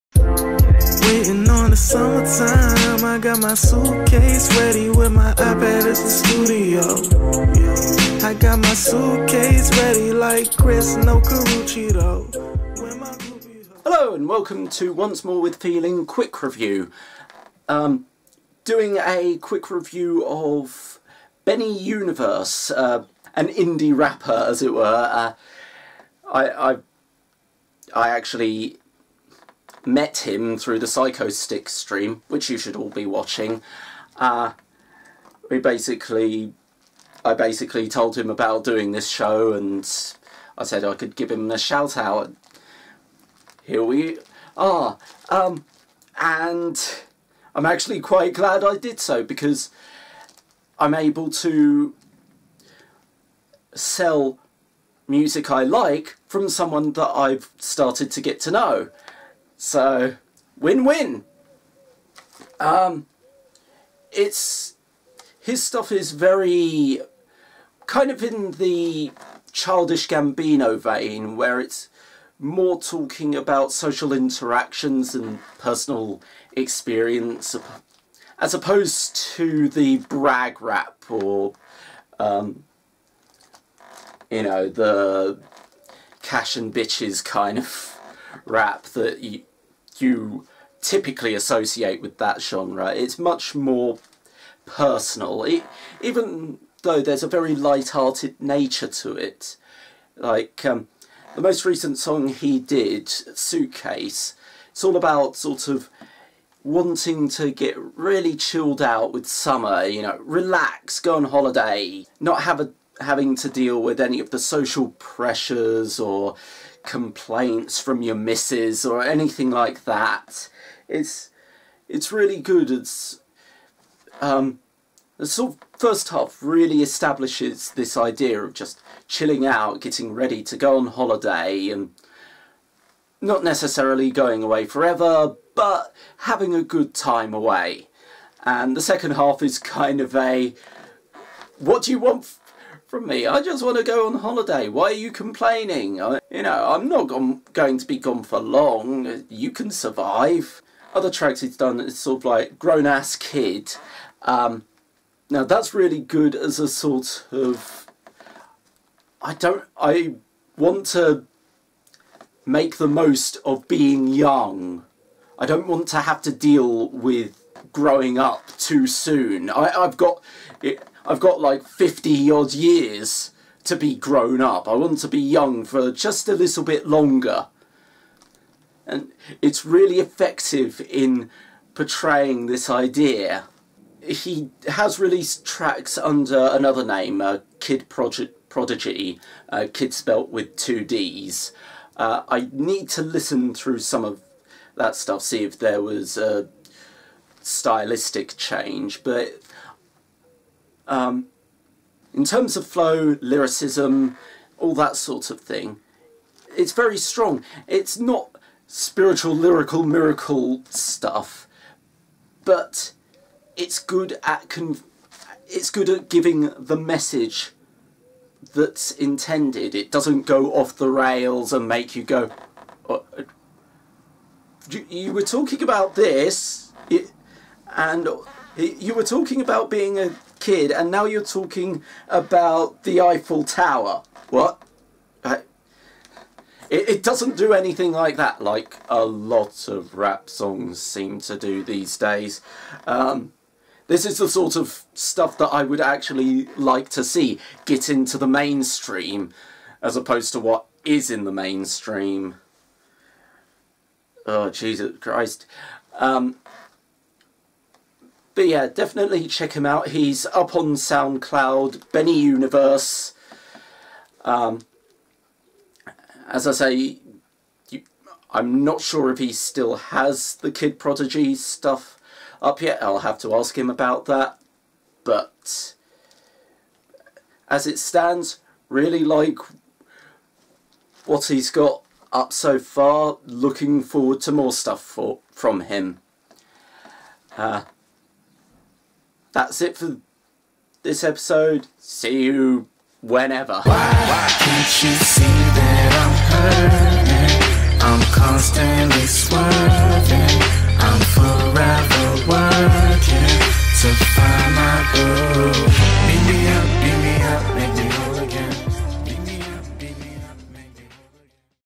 Quick Review/Artist Showcase - Benny Universe
Doing a quick shoutout to an up-and-comer in the rap game.